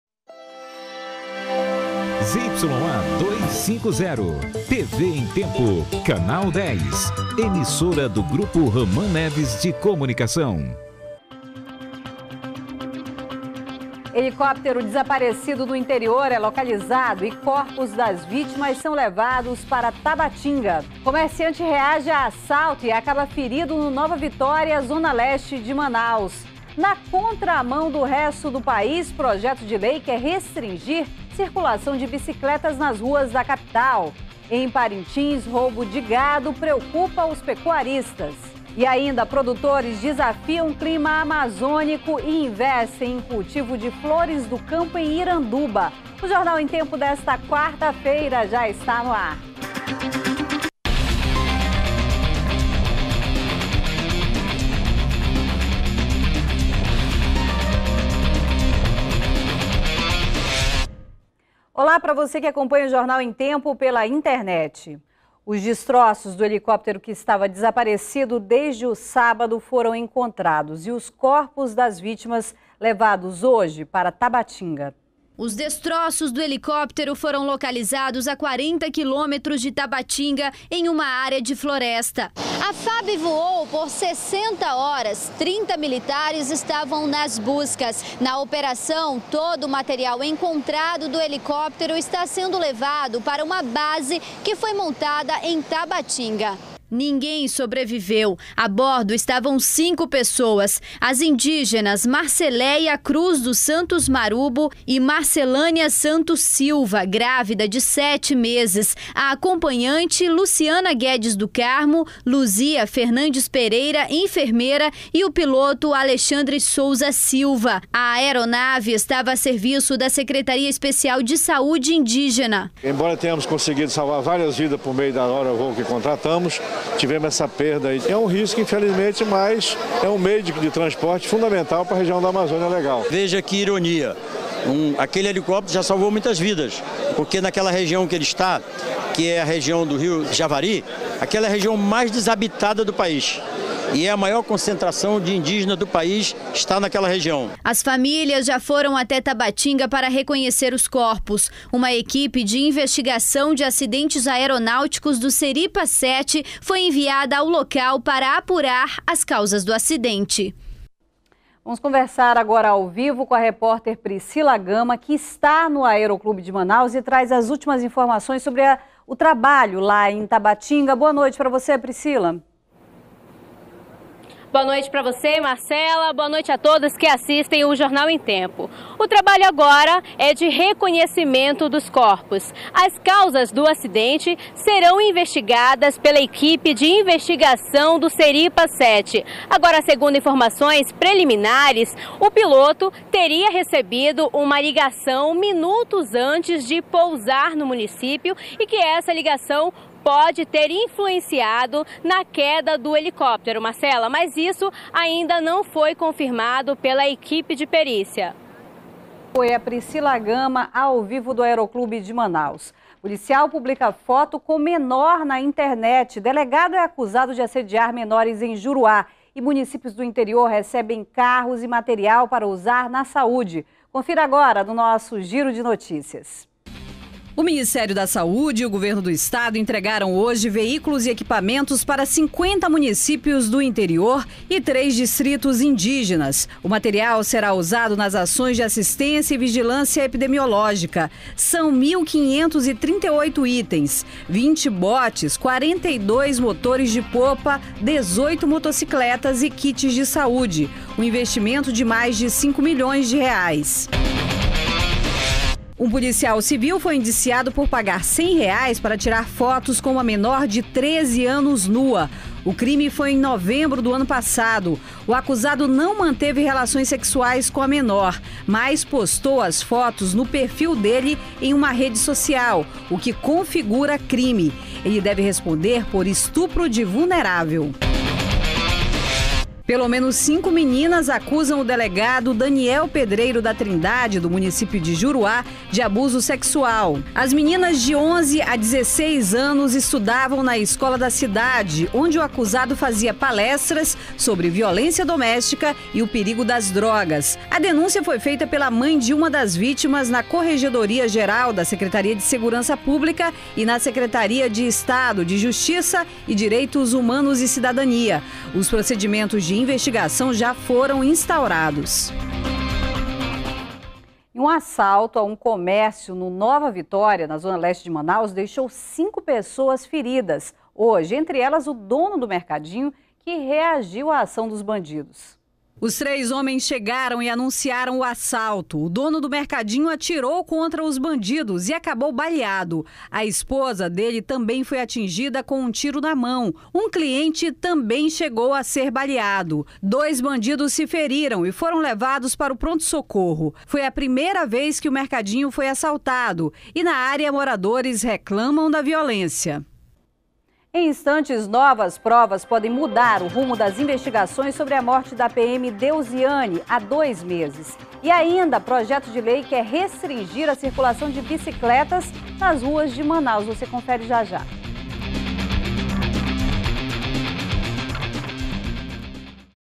Comerciante reage a assalto e acaba ferido no Nova Vitória
01º BLOCO JORNAL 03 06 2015 - Comerciante reage a assalto e acaba ferido no Nova Vitória